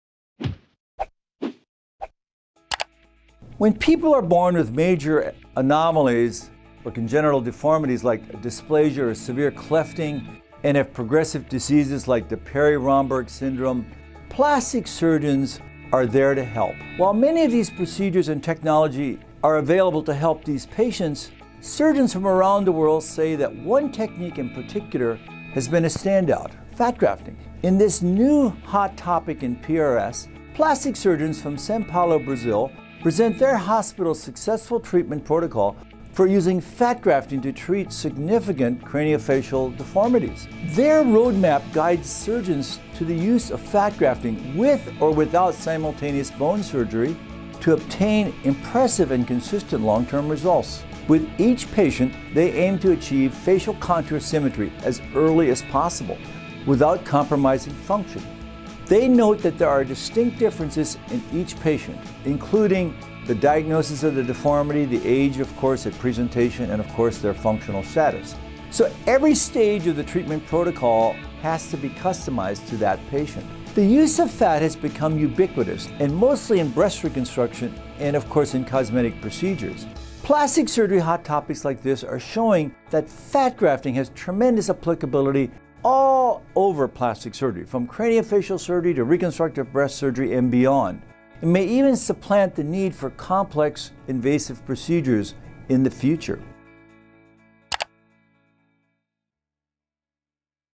Fat Grafting Magic. Plastic Surgery Hot Topics with Rod J. Rohrich, MD
Though many procedures are available to help patients with craniofacial deformities, surgeons from around the world say one technique in particular has become essential: FAT GRAFTING.

In this video, Rod J. Rohrich, MD, Editor-in-Chief of “Plastic and Reconstructive Surgery,” discusses a roadmap that guides surgeons’ use of fat grafting to obtain impressive, consistent results for these patients.

Read the May 2019 PRS PlasticSurgery article, “Fat Grafting in Managing Craniofacial Deformities” by Denadai, Raposo-Amaral, and Raposo-Amaral.